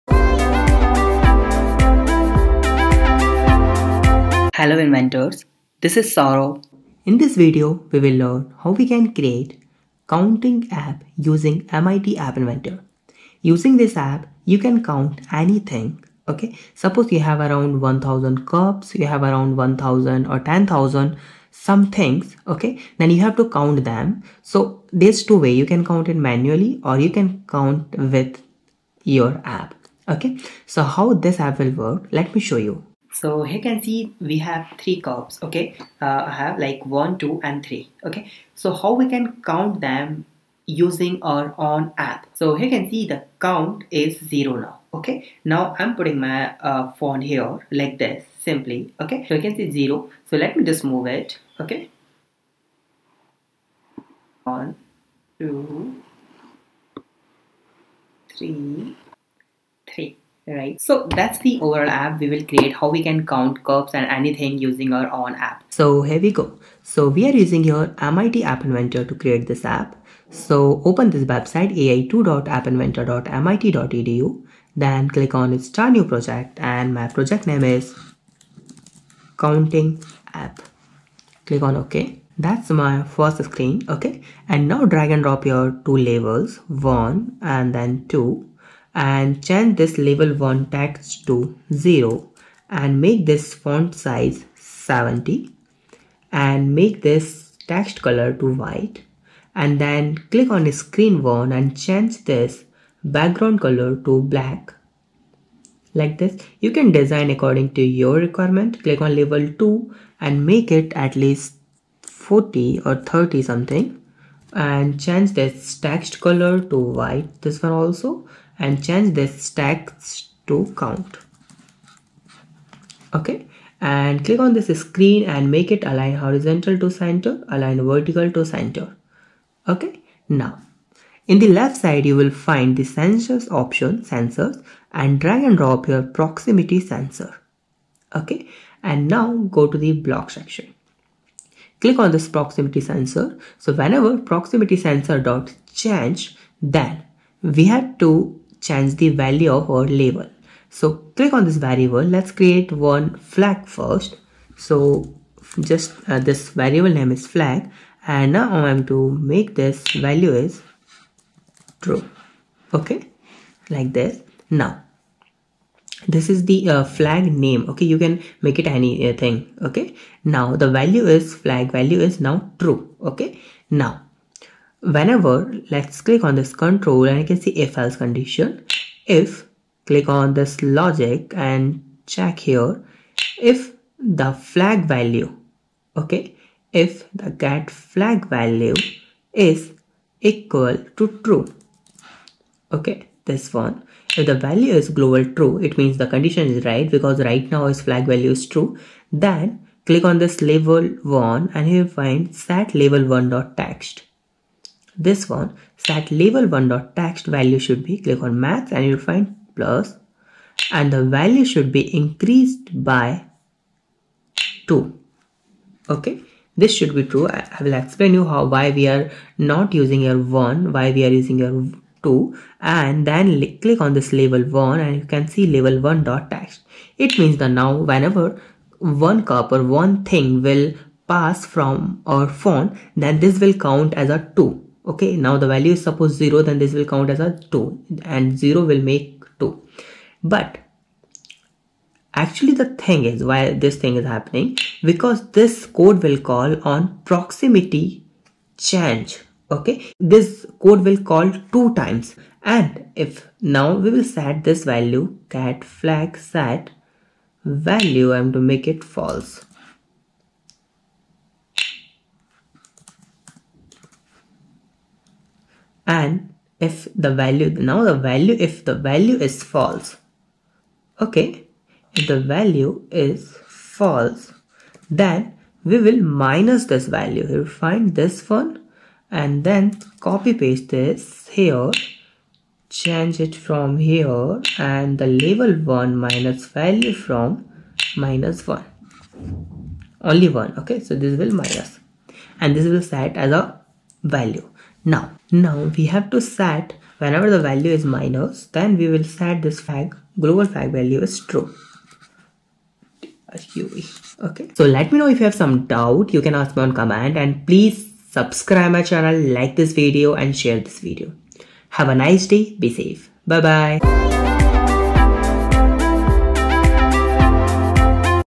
How To Make Auto Counting Robot App in MIT App Inventor | AI App
In this video, I'm going to show you how we can make an app that can count things with the help of our android app sensor. Using this we can have a simple robot in our factory or school that can count the labor, students, or goods of the factory.